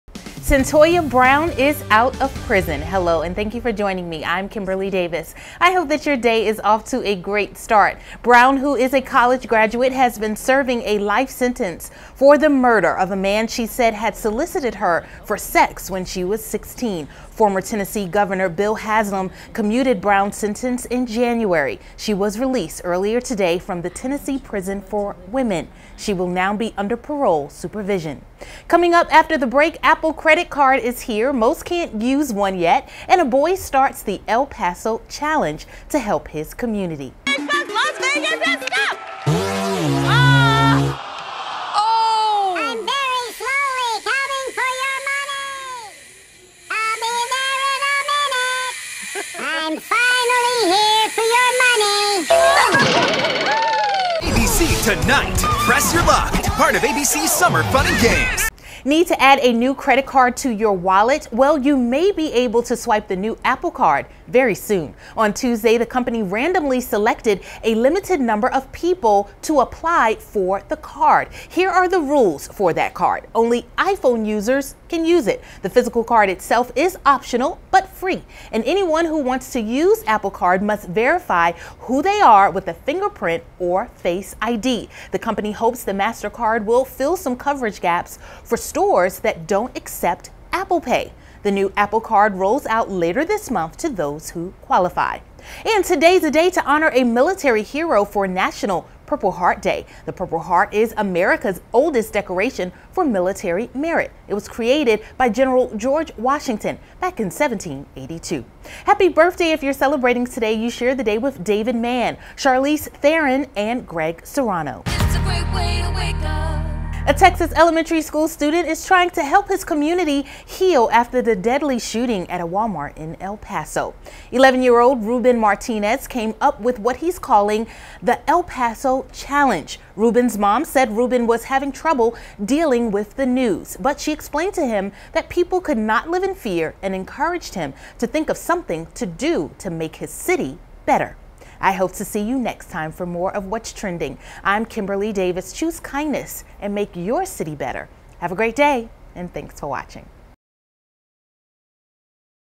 Trending: Cyntoia Brown released from prison and Texas boy starts El Paso Challenge
Press ▶️
on ABC Columbia
- CyntoiaBrown released from prison
- AppleCreditCard is here, for some
- TX boy starts ElPasoChallenge to help community
- Wednesday is National
🎉 David Mann
🎉 Charlize Theron
🎉 Greg Serano